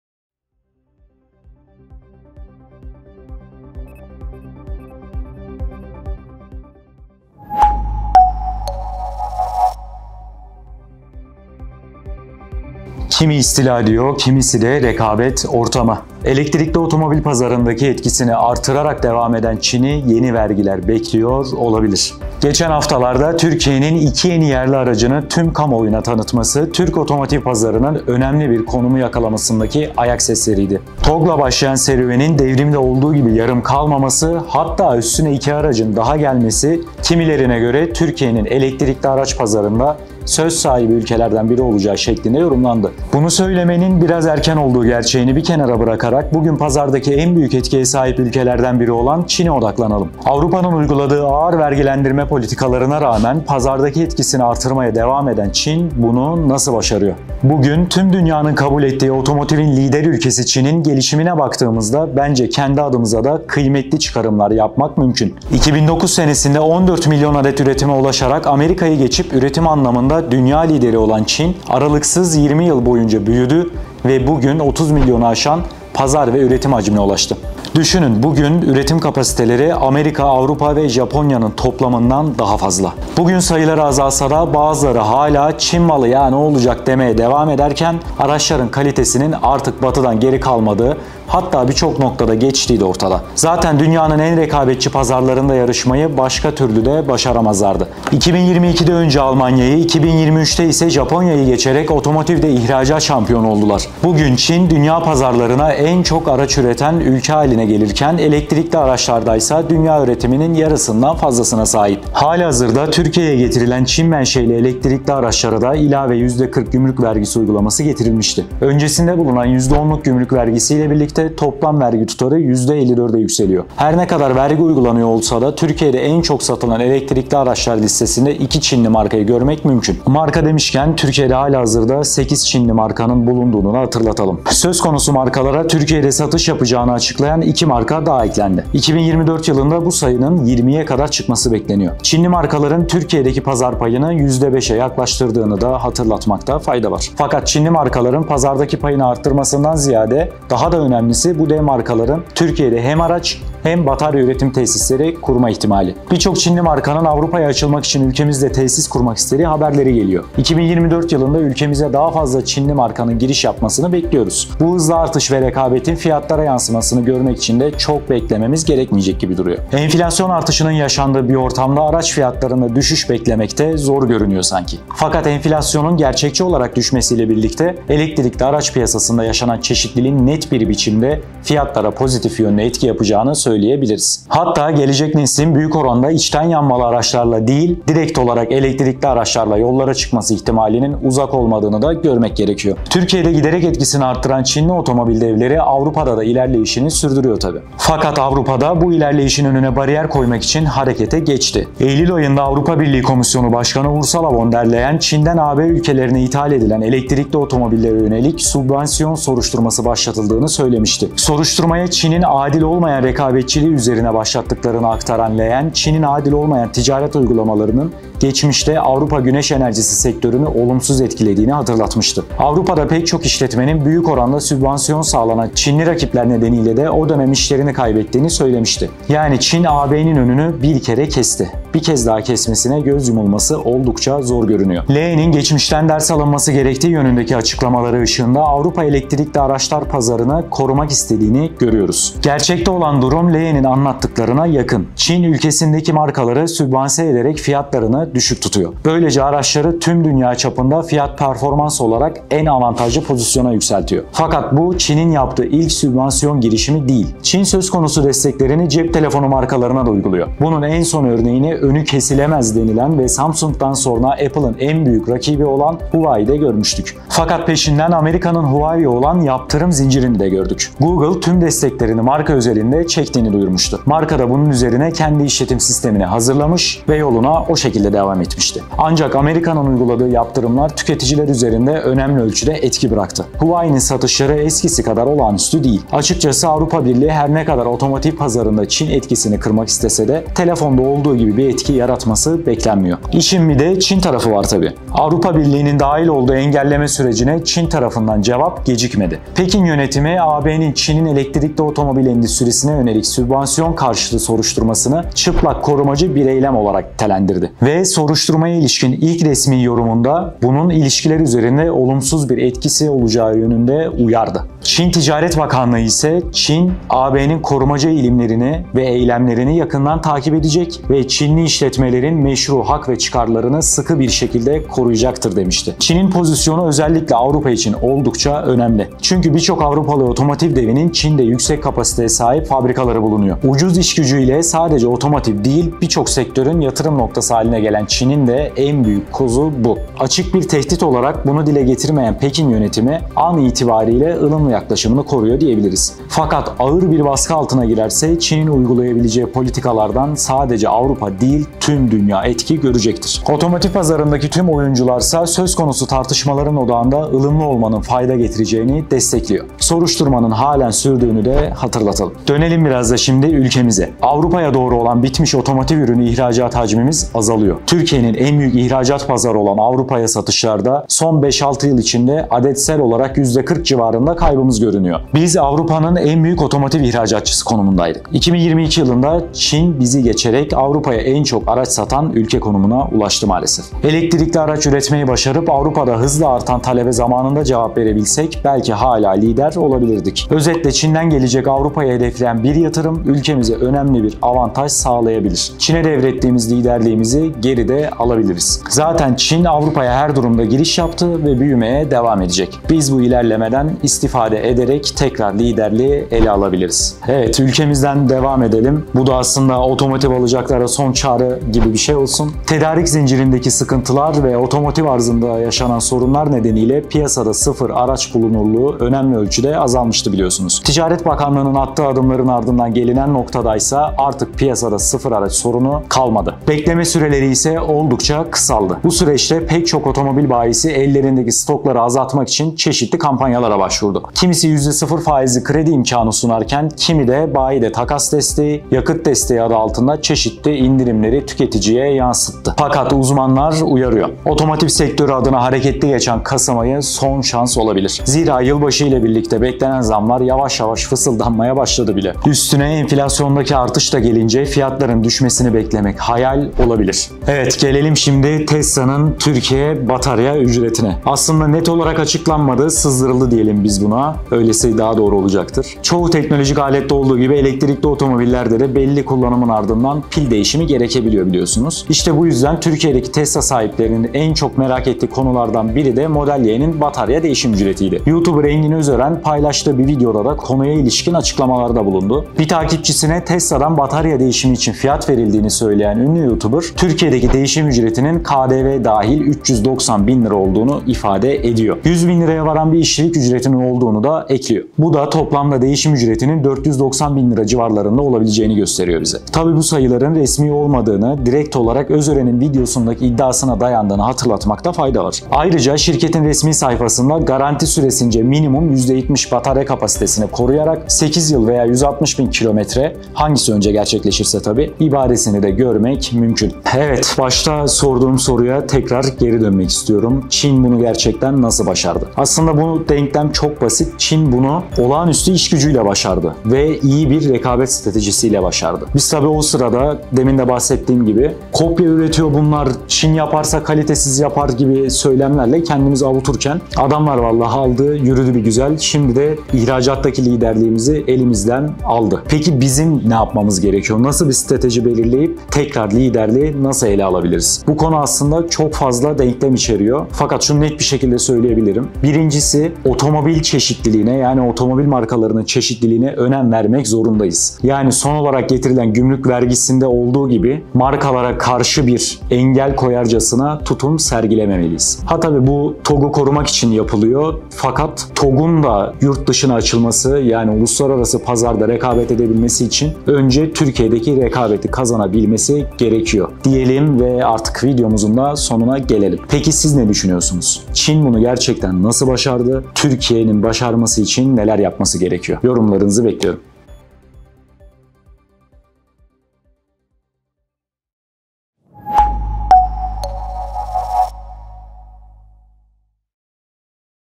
Otomobil Alacaklara Son Çağrı! Çin etkisi vergilerle karşı karşıya...
Kimi istila diyor kimisi de rekabet ortamı…
Elektrikli otomobil pazarındaki etkisini artırarak devam eden Çin’i yeni vergiler bekliyor olabilir.